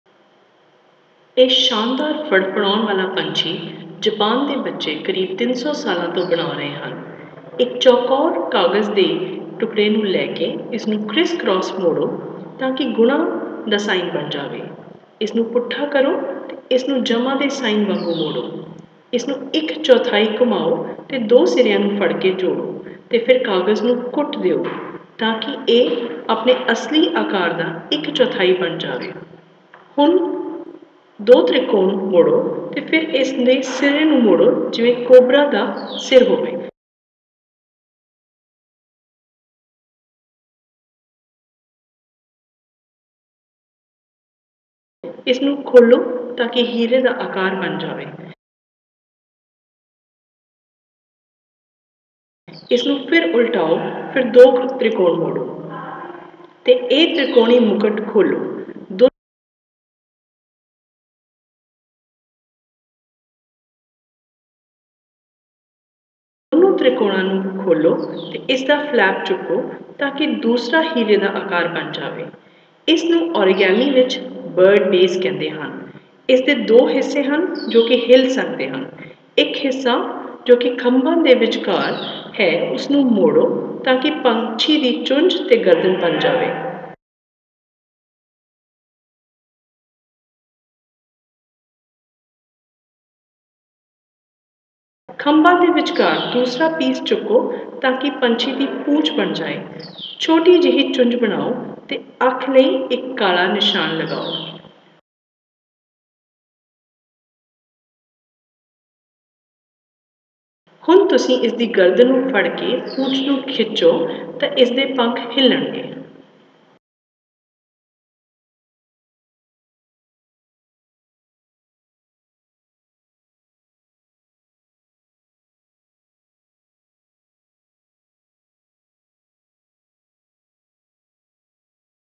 FLAPPING BIRD | Punjabi
This marvelous Flapping Bird has been made by the children in Japan for the last 300 years. Start with a paper square. Fold cress and a cross, like a multiplication sign. Upturn the paper and fold a minus sign. Then rotate the paper by a quarter and fold a plus sign. Then squash and fold the paper so that it becomes a square – quarter in size of the original one.
Fold two triangles along the diagonal. Then fold the top crown. The model will look like a snake’s head. Lift it to the base of the crown and make a diamond shape. Reverse the paper. Once again fold two triangles to the diagonal and then fold the crown. Lift the flap to make the second diamond. In Origami this shape is also called the BIRD BASE. The two ends at the bottom are not joined and can move. Lift one end between the wings to make the neck and the beak. Similarly, lift the other end to make the tail of the bird. Place a black dot for the eye.
Hold the neck with one hand and push-pull the tail to make the bird flap its wings.